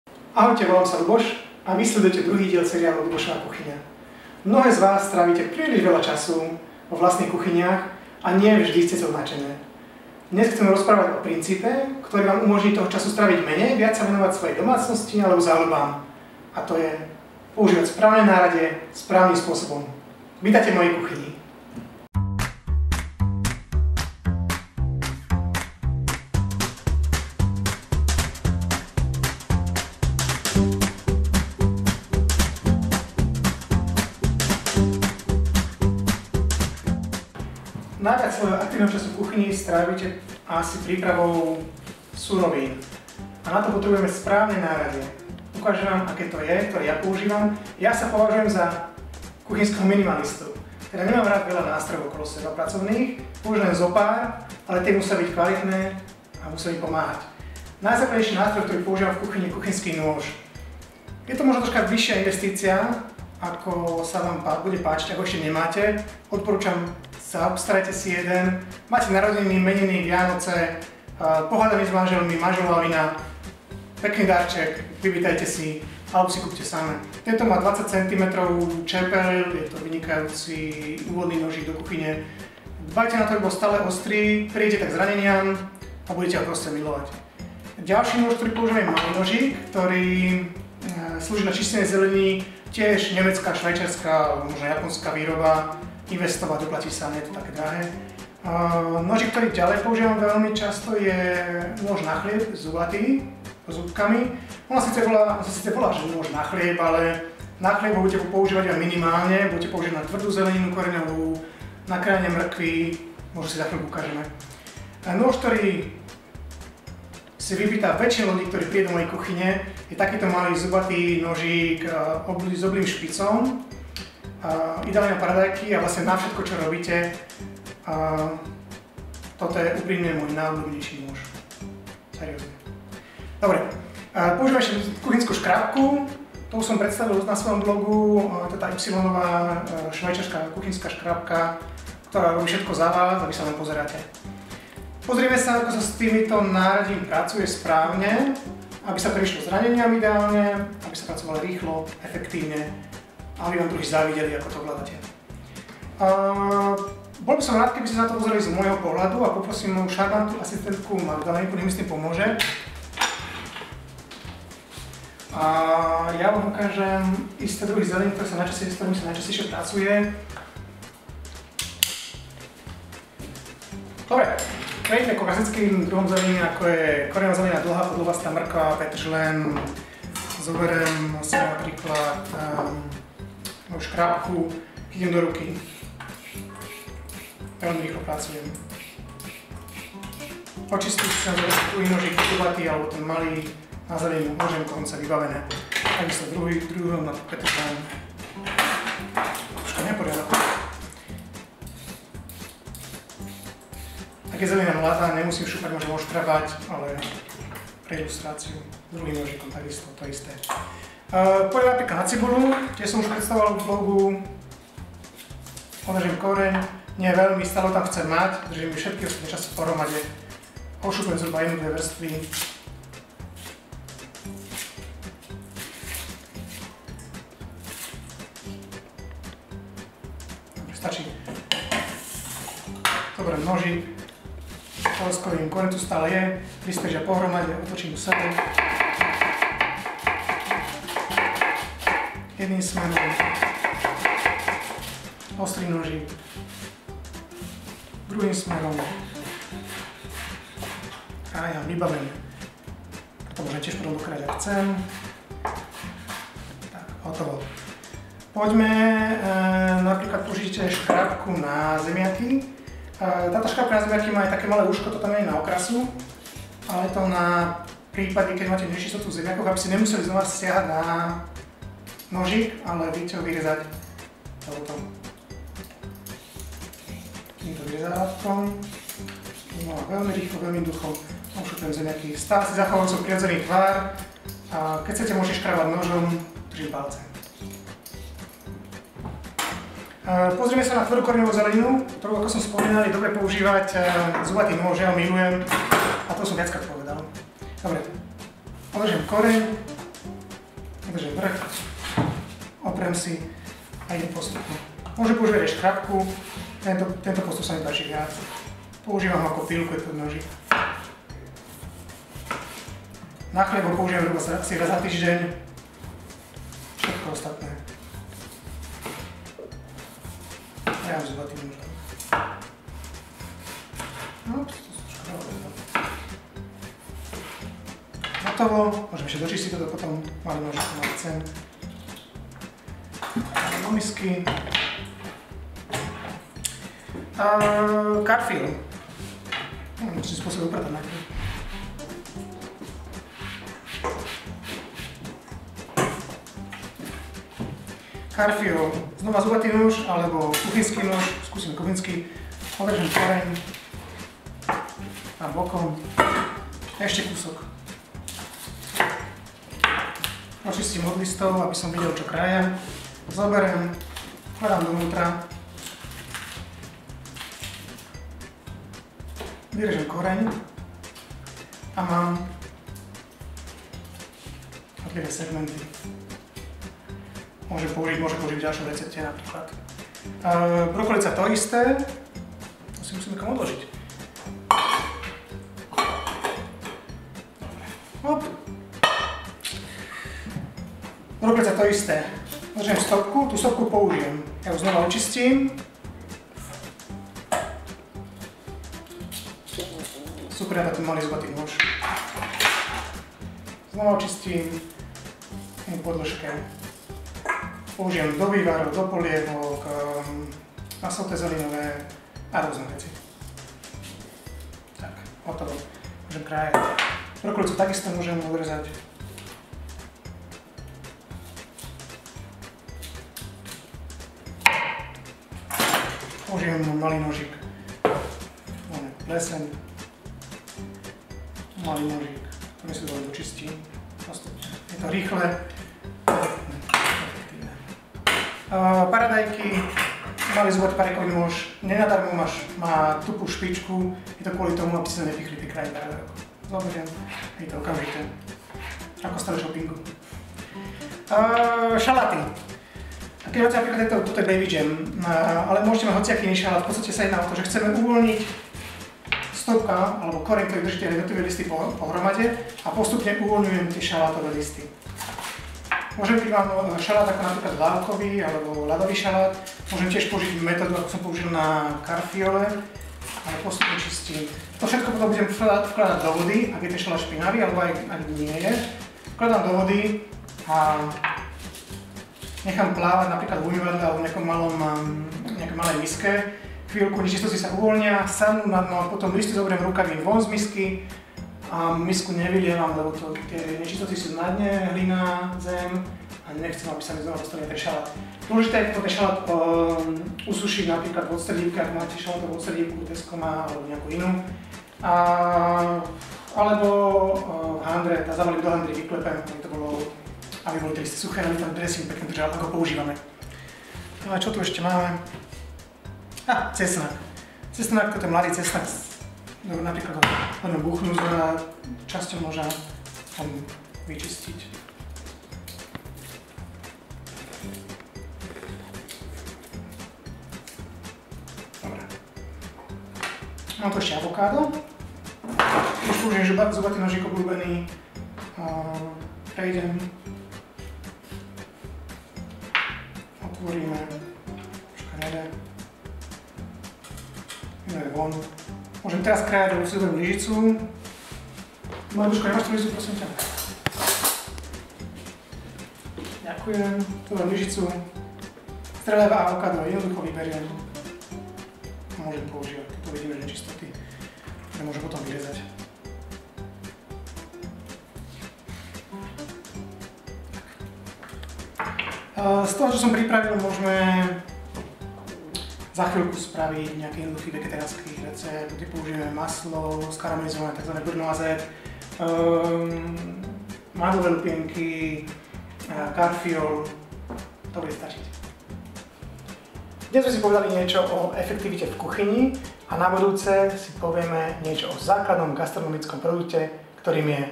Ľubošova kuchyňa #2: Efektivita
Ahojte! Máme tu druhý diel seriálu Ľubošova kuchyňa, tentoraz na tému produktivity v kuchyni.
Verím, že samotné varenie môže byť vcelku pasívny proces a najviac aktívneho času pri varení strávime väčšinou prípravou surovín (nepočítam nákupy a umývanie riadov). Nasledujúce video vám v skratke prináša moje skúsenosti z prípravy jedál získané počas rokov strávených v kuchyni. Budem rád, ak vás niečo zaujme...
Dobrú chuť!  S pozdravom, Ľuboš.